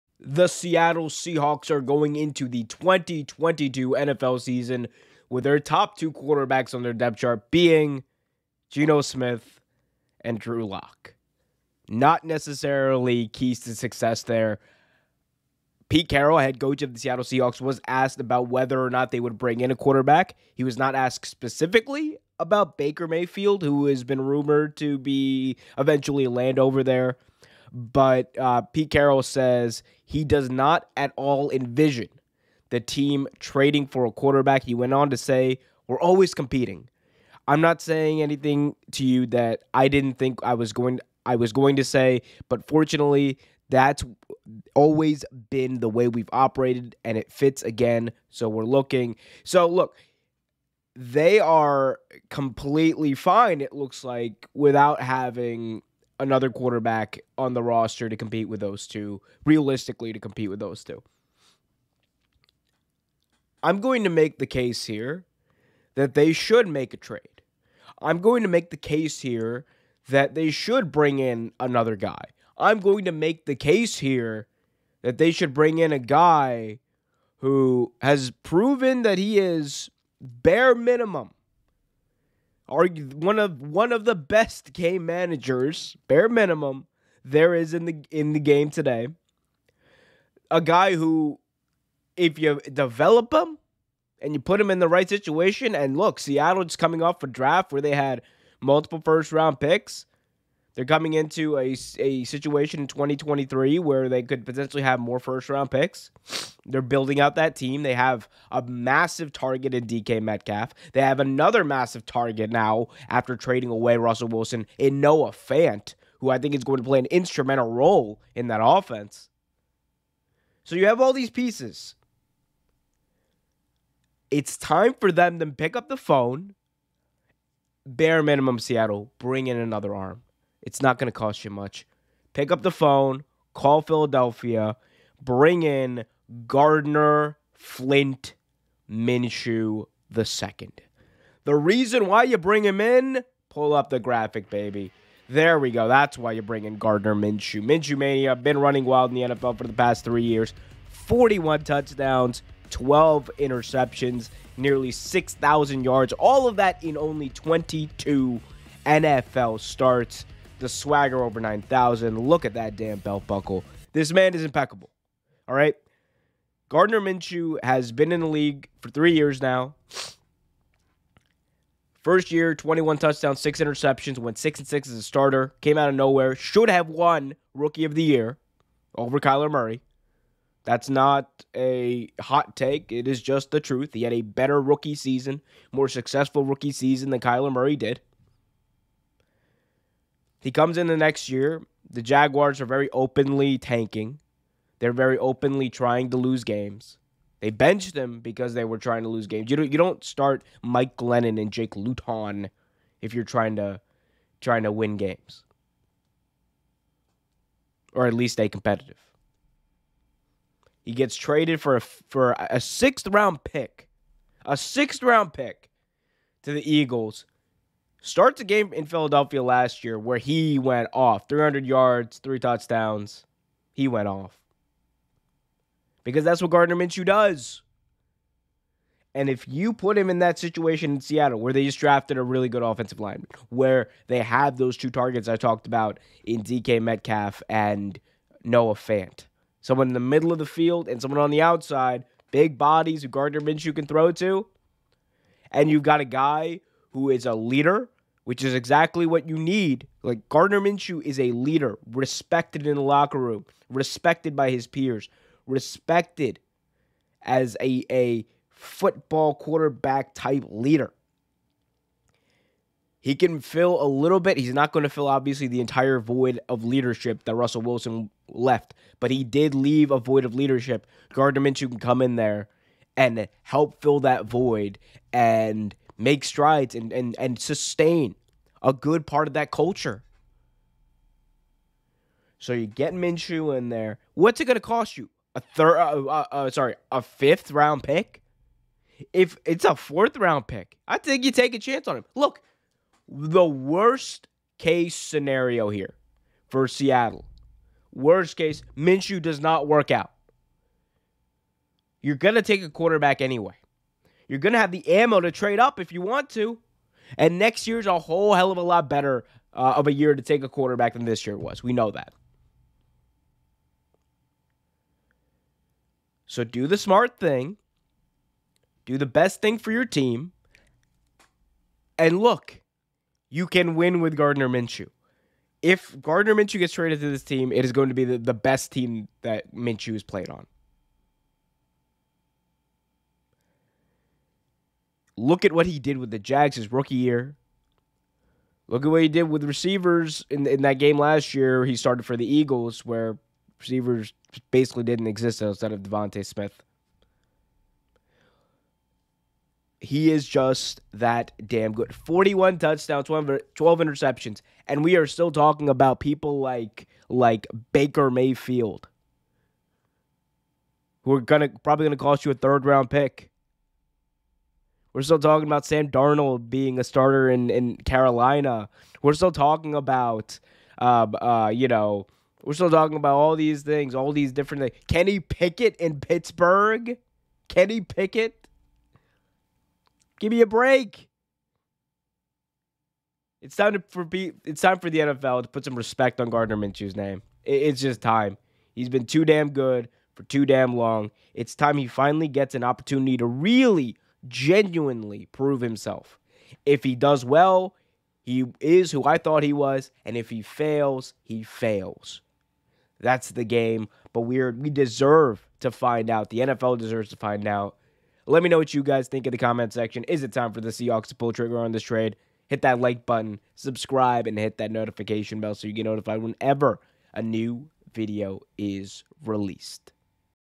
The Seattle Seahawks Should TRADE For Gardner Minshew
The Seattle Seahawks are going into the 2022 NFL season with an opening at quarterback and we make the case for them to go after Gardner Minshew